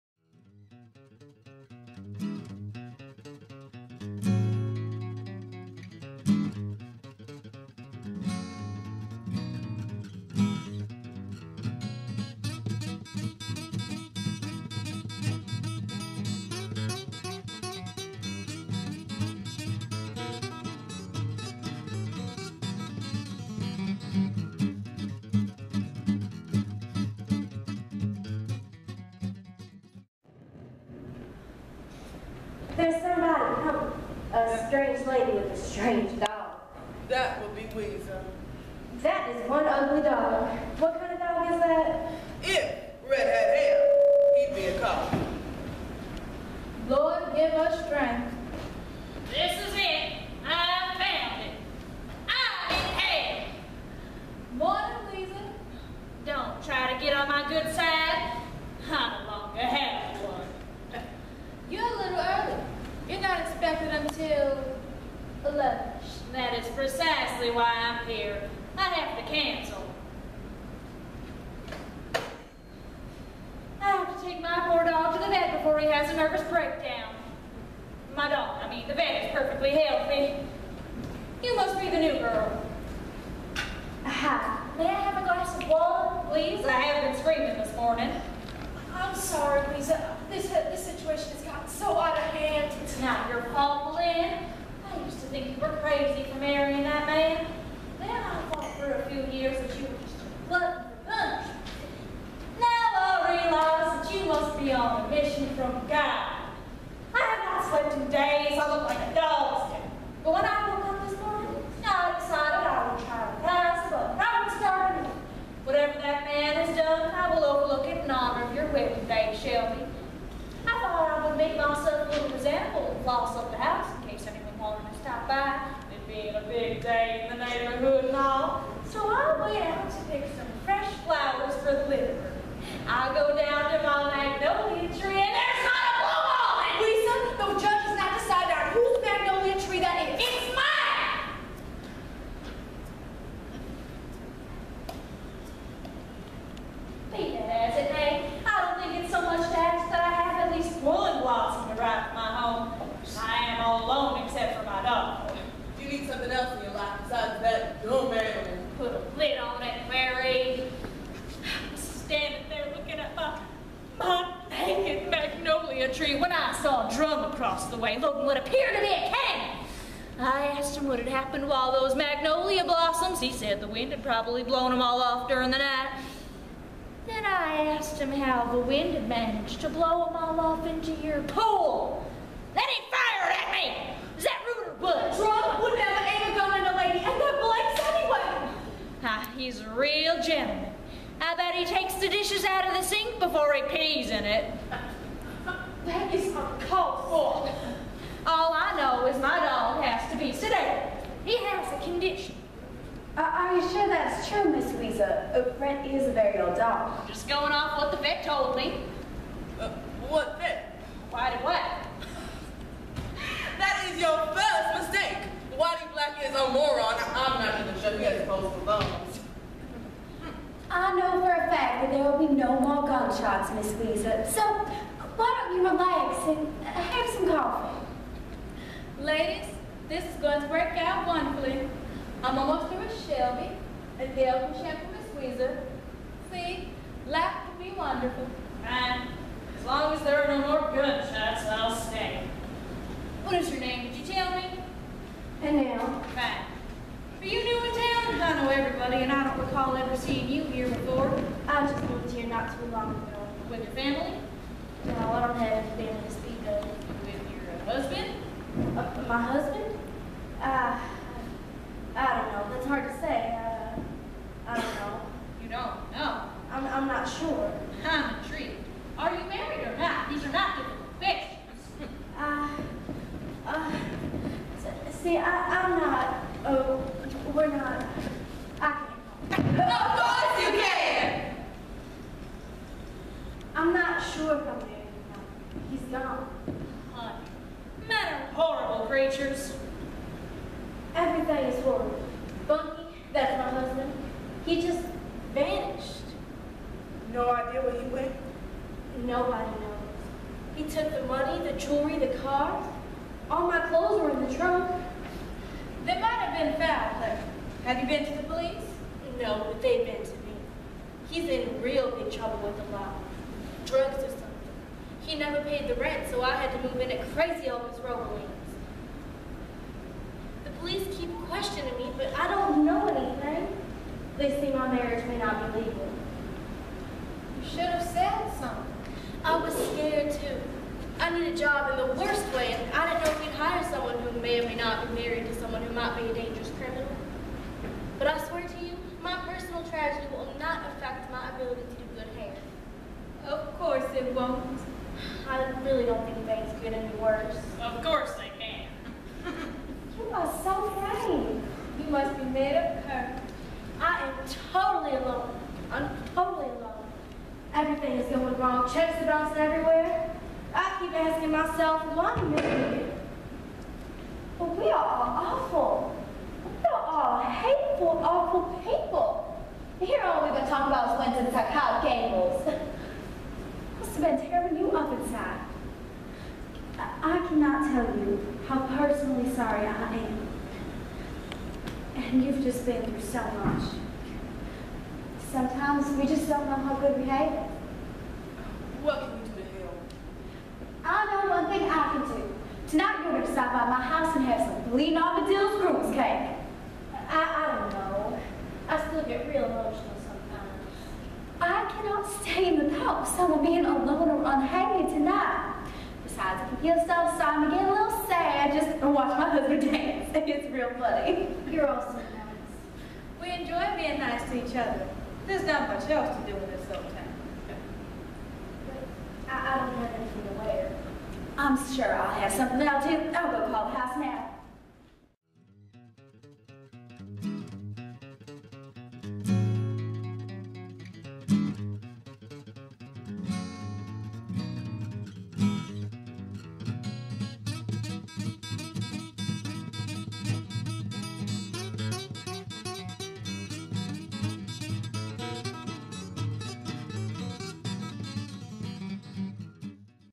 ddeFallPlay15
Eisenhower High School will present "Steel Magnolias' as its fall play on Nov. 12, 13 and 14 at 7 p.m.  Tickets are $7 for adults and $5 for students with IDs and senior citizens.  Here's a great scene!